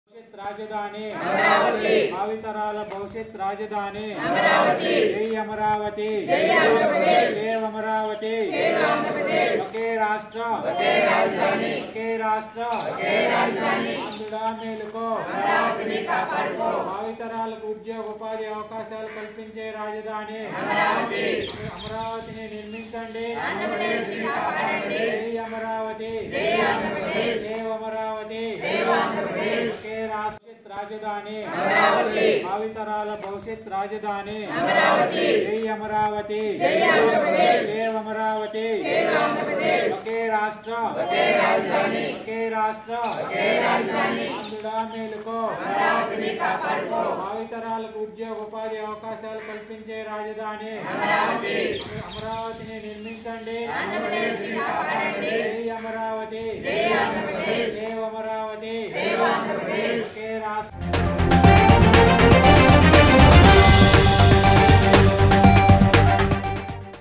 1072వ రోజు తాడికొండ దీక్షా శిబిరం.. అమరావతి ధర్మ పోరాటం మహాధర్నా కార్యక్రమం | Amaravati Galam
1072వ రోజు తాడికొండ దీక్షా శిబిరం.. అమరావతి ధర్మ పోరాటం మహాధర్నా కార్యక్రమం | Amaravati Galam